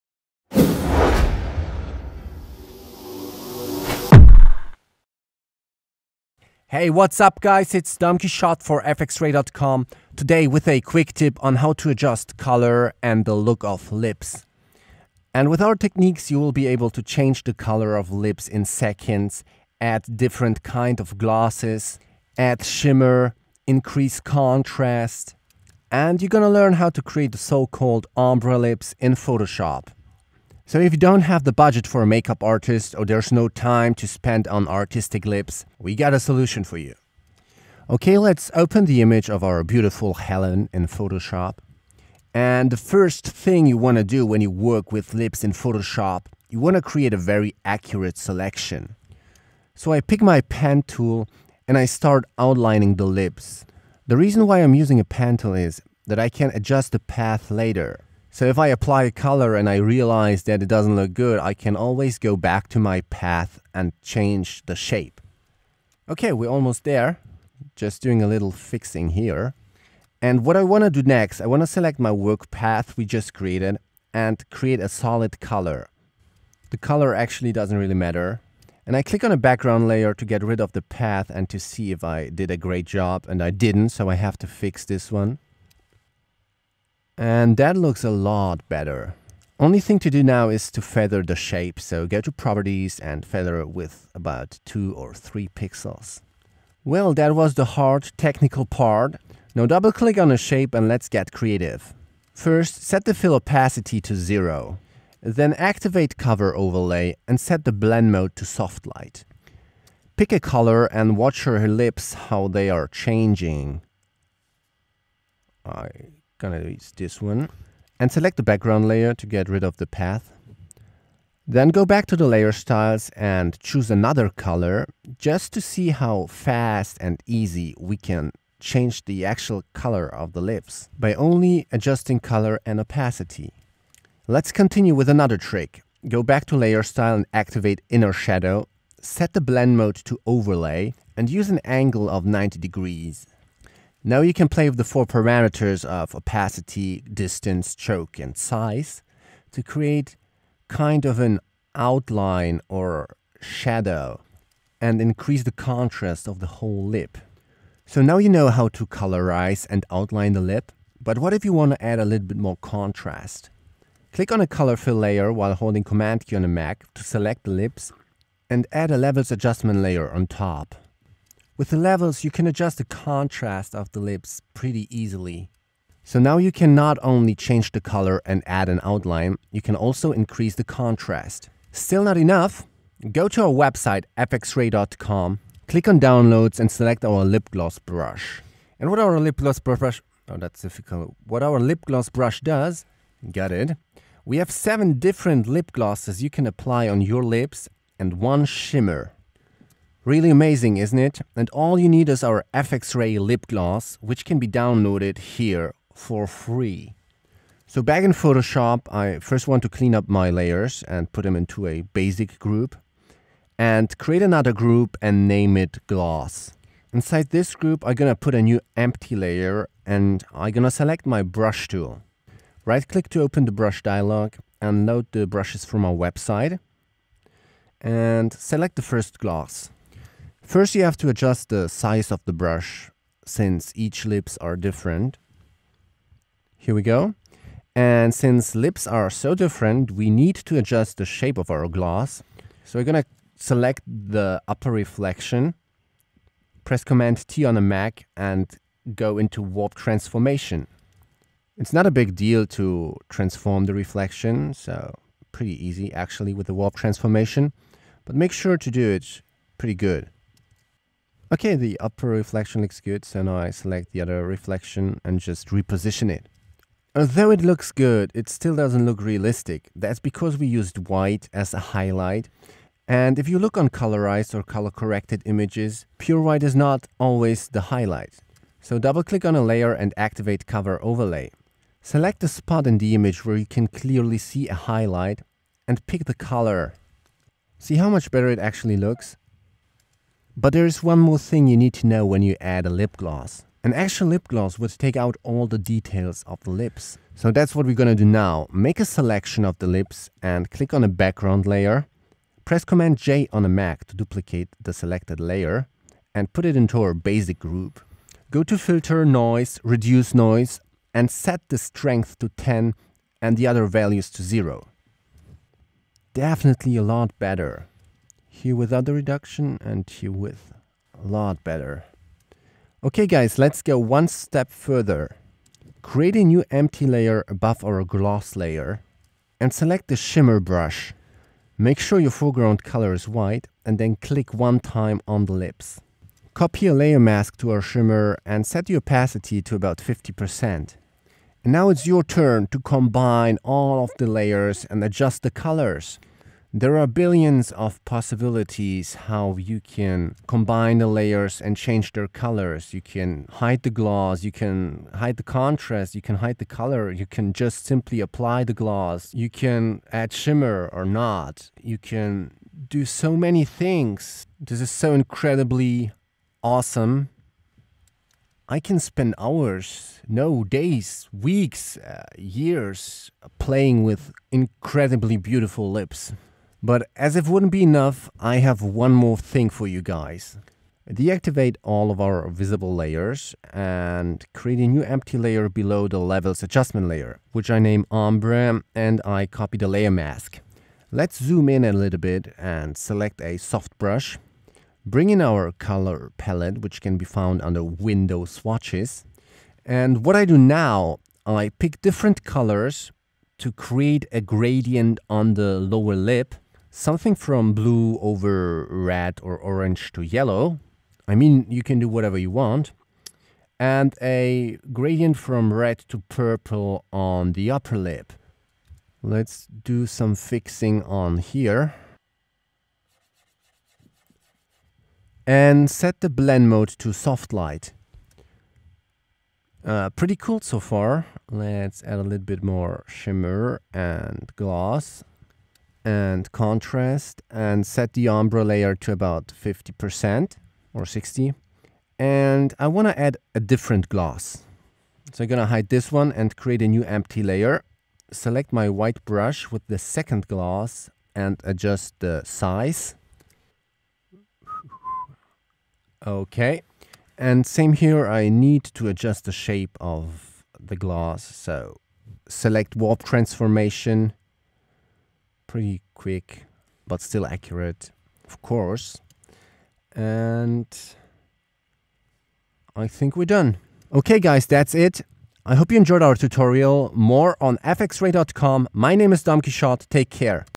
Photoshop Quick-Tip: Lips, Glossy Lips, Ombre Lips
Change the color of lips, add gloss, shimmer and make ombre lips